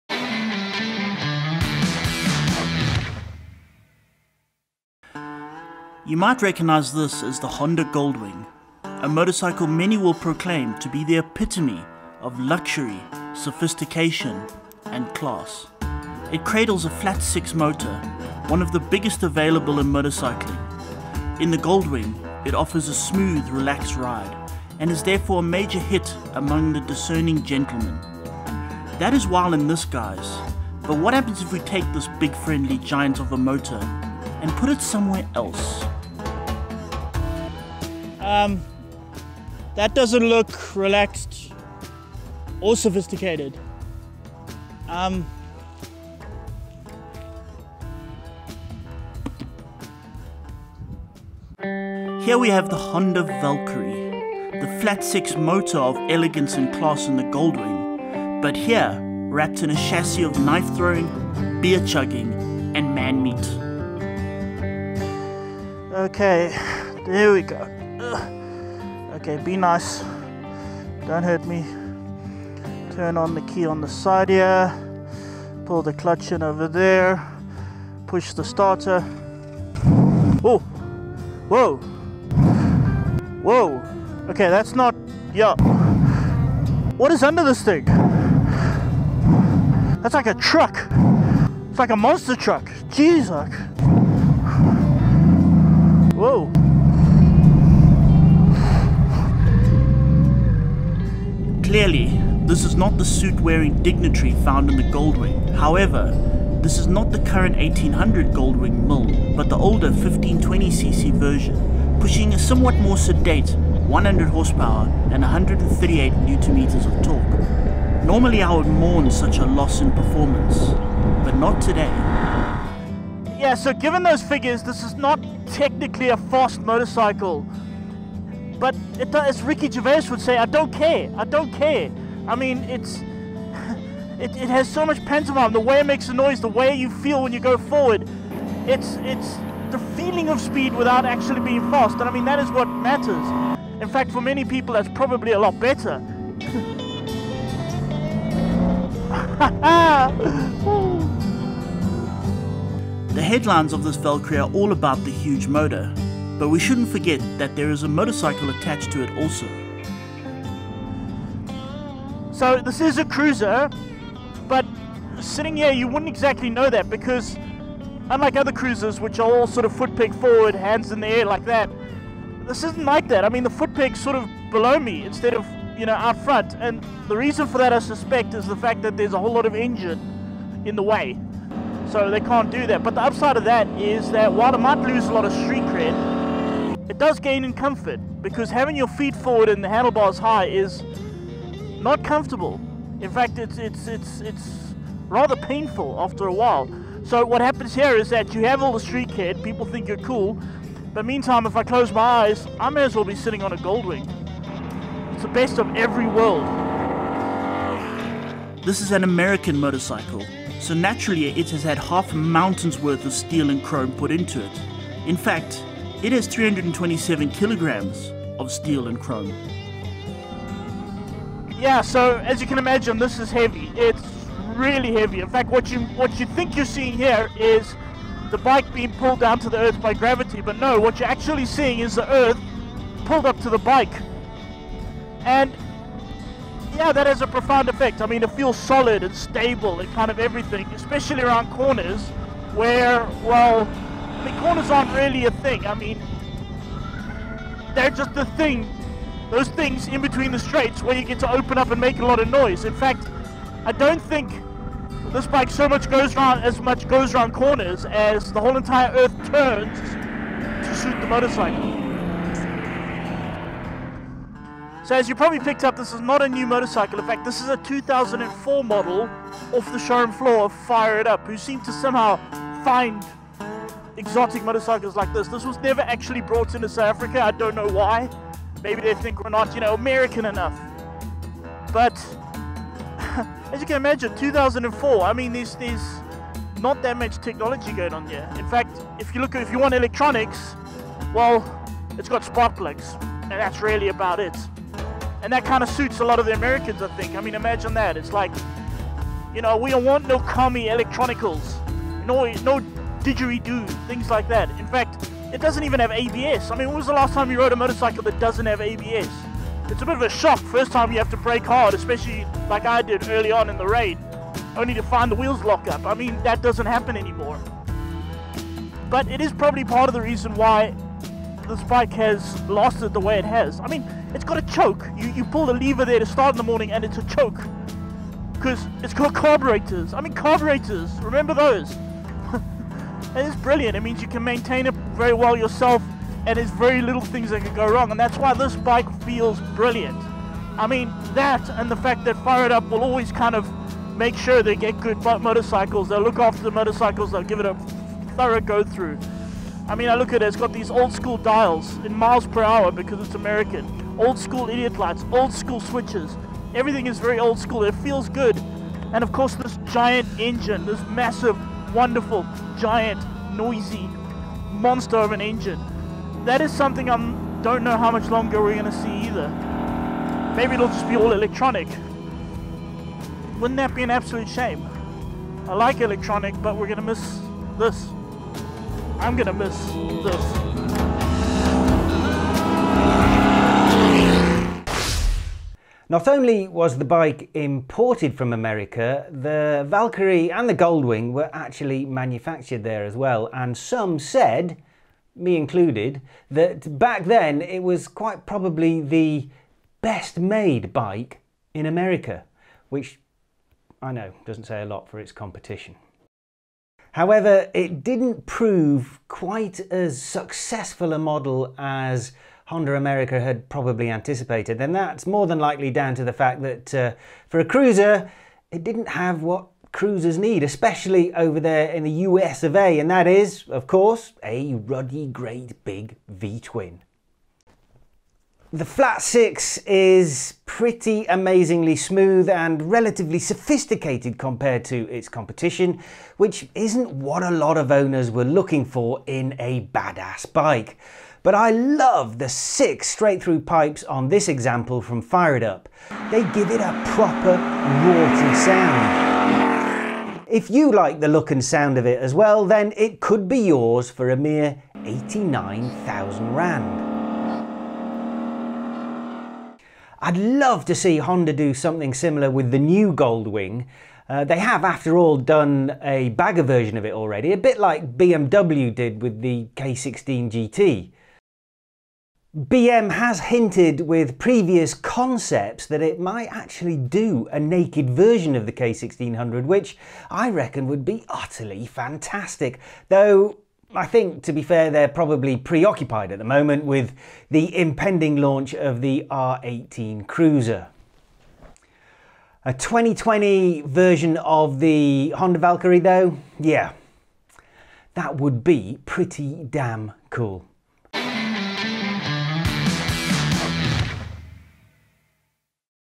The Mad Honda Valkyrie
The Honda Gold Wing is famous for its luxurious elegance. But what happens when we take that smooth flat-six motor and give it to the black sheep of the family? Welcome to the Honda Valkyrie.